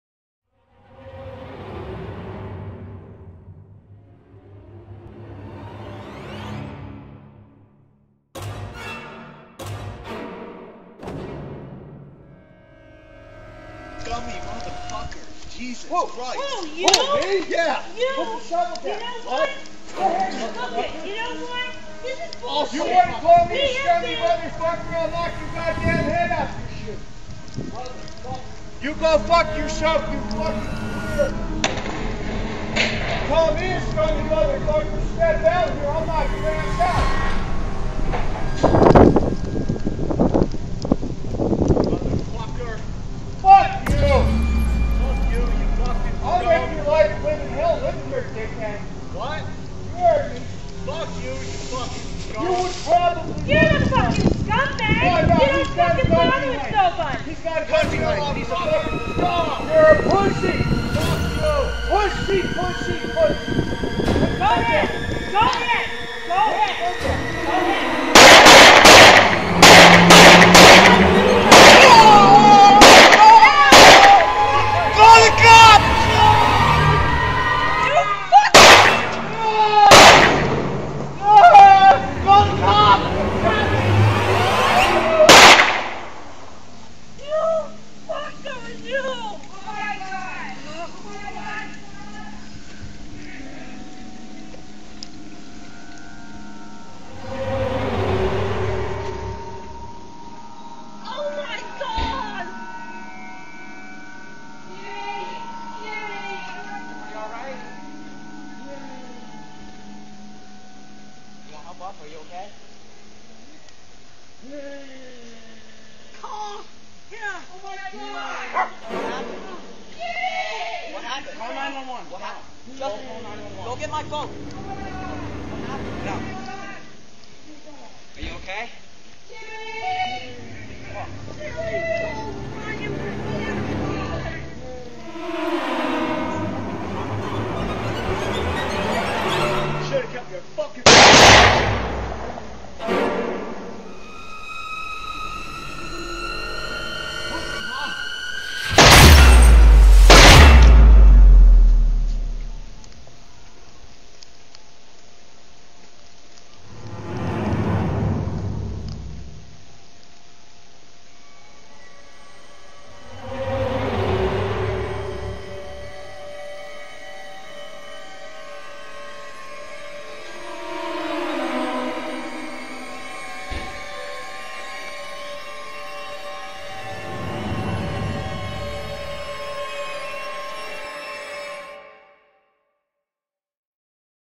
MUTANTLAB : MUTANTMURDER
Animutation of the February 1st 2021 Pennsylvania snow shoveling murder suicide shooting incident involving Jeffrey Spaide, James and Lisa Goy.